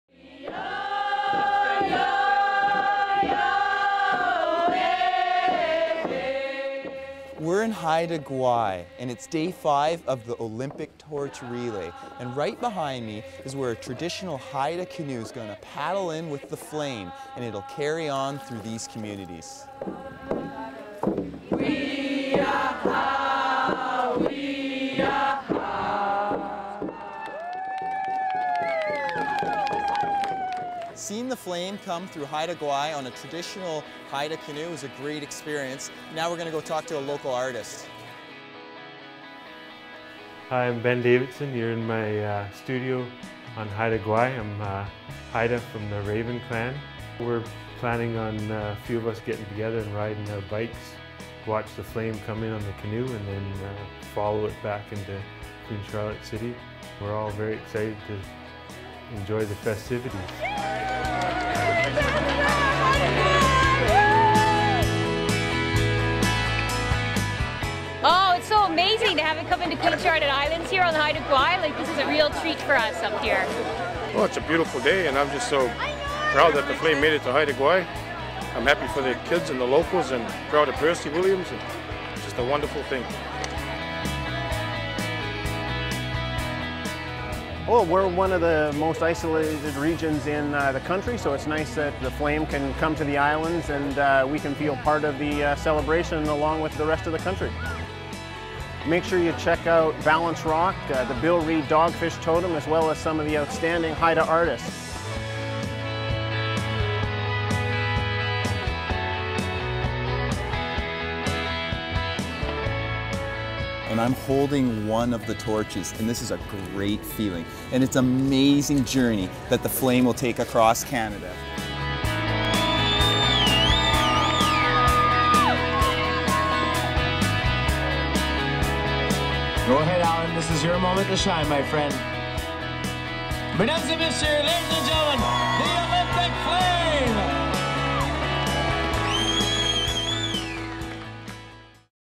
Day 5 Olympic Torch Relay: Haida Gwaii, British Columbia
Chris Wheeler follows the Olympic Torch Relay to Haida Gwaii, British Columbia, and meets up with the locals to enjoy the festivities.